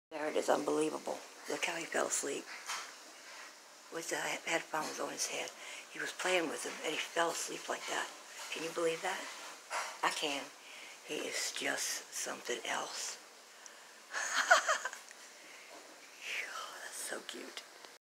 He Falls Asleep to Music (my ferret TootieBug)
He actually falls asleep like this.